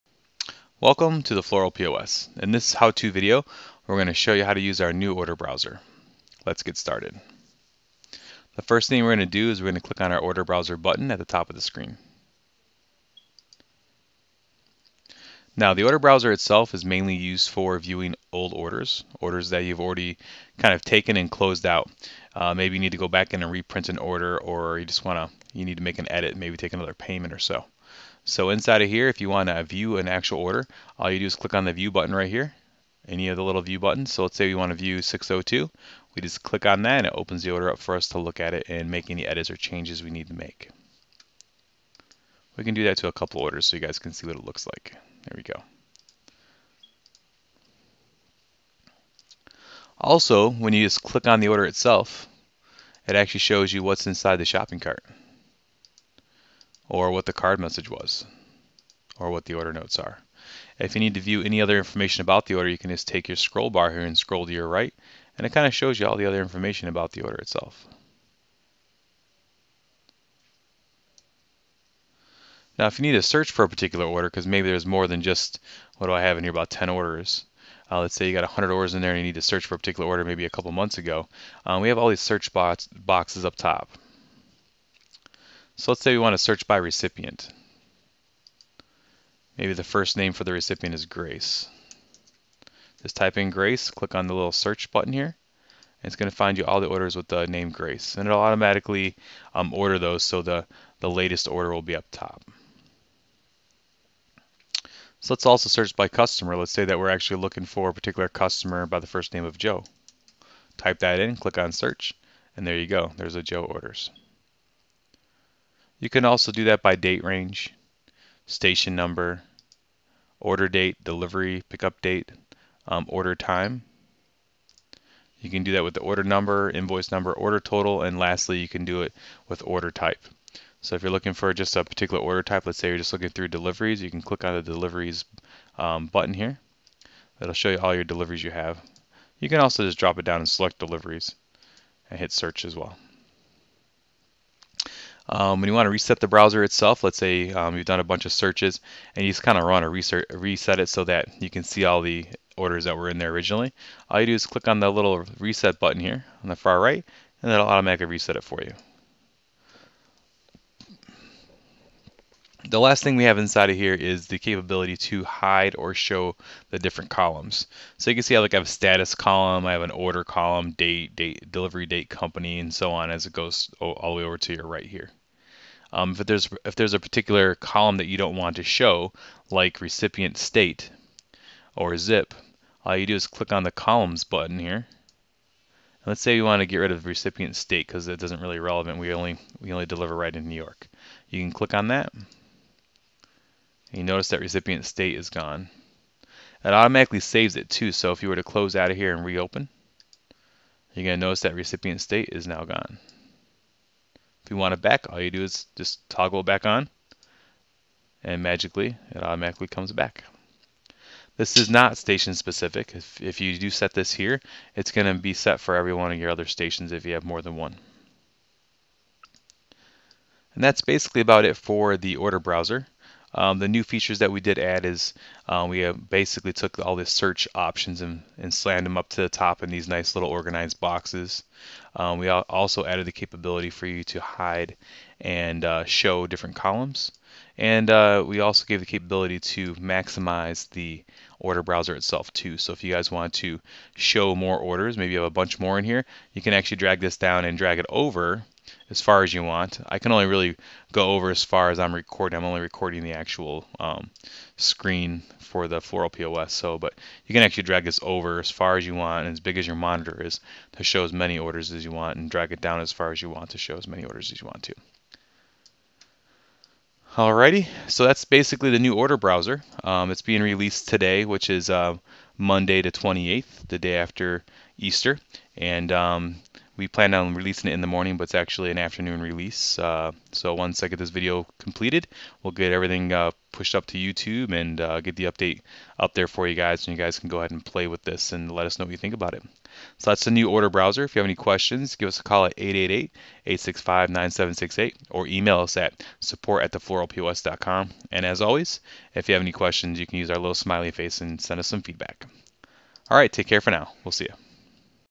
How To - Use The Order Browser
Our Order Browser allows you to preview, view, search for orders in the system. We now allow Hide / Show of all Columns as well as Sizing of the Order Browser to fit more orders.